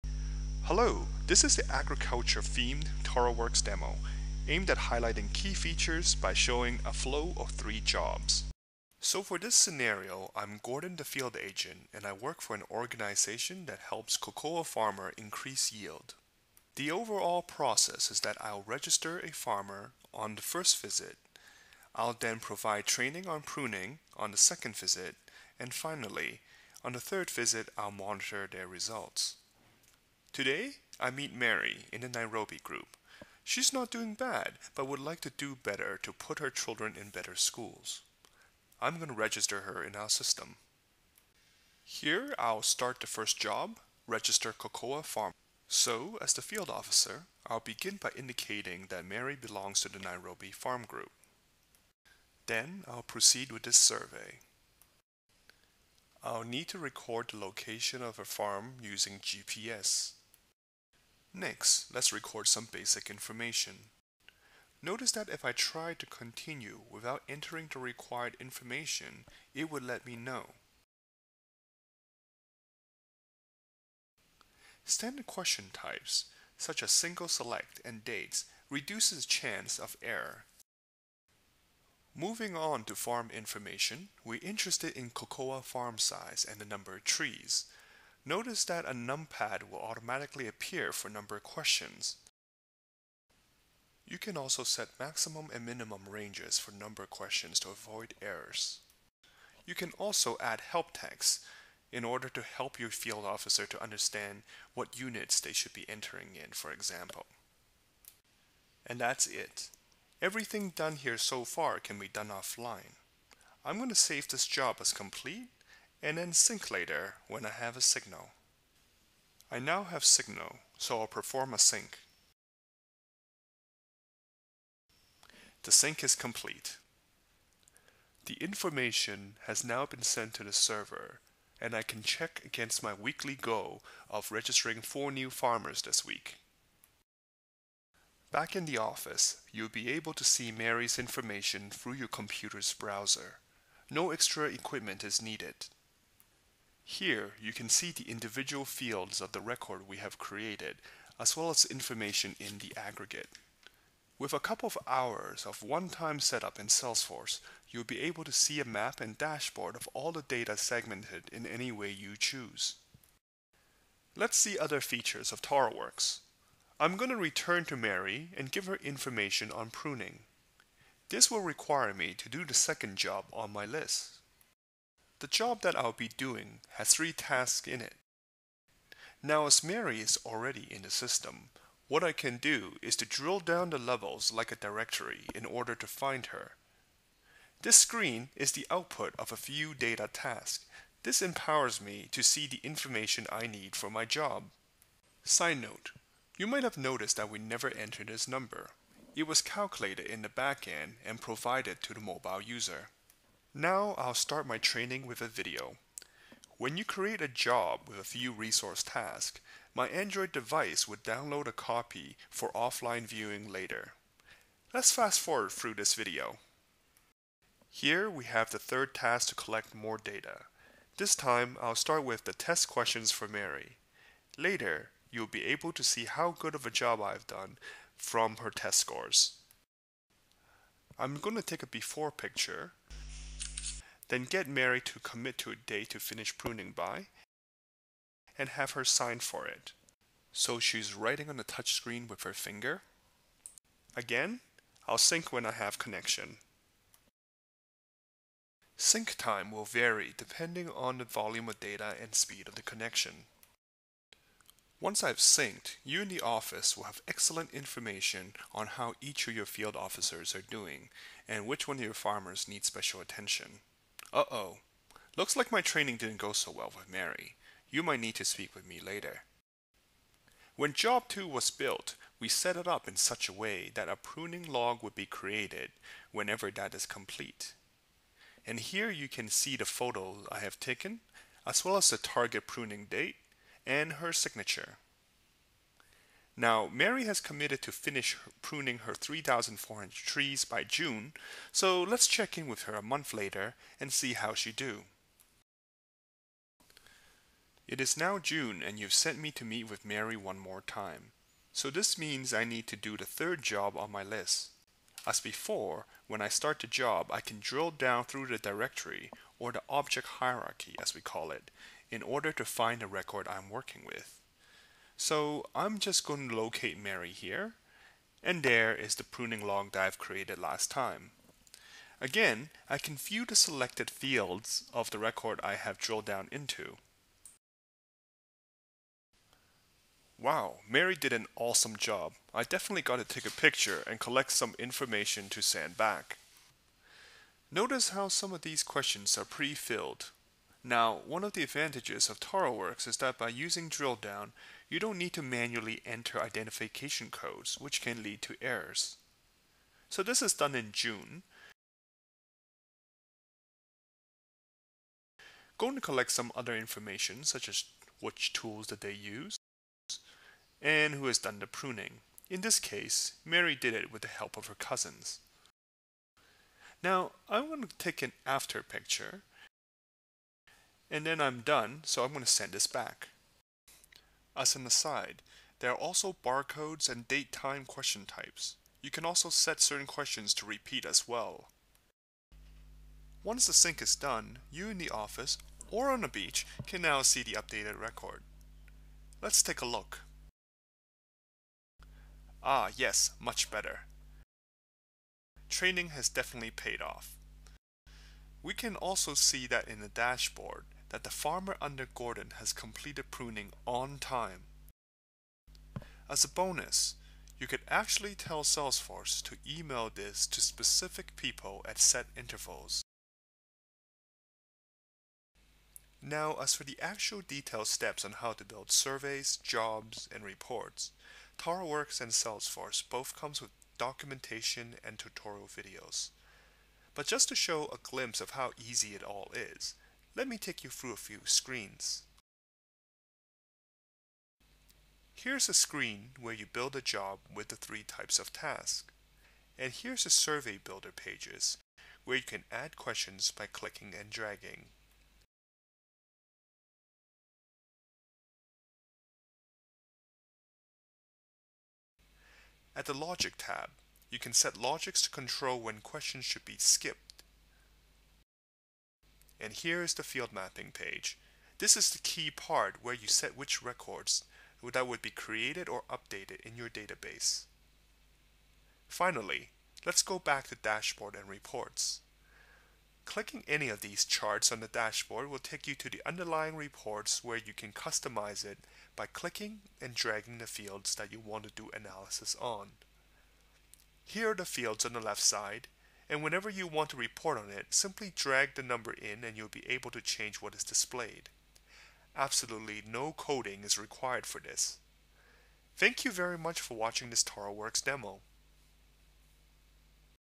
Expanded Demo End-to-End TaroWork Operations
This is an expanded demo showing how you can use TaroWorks to register a stakeholder, collect information, do multimedia training and empower your field staff with information from your central database.

While this is an agriculture scenario you can easily make any system tailored to your operations. To learn more, please visit taroworks.org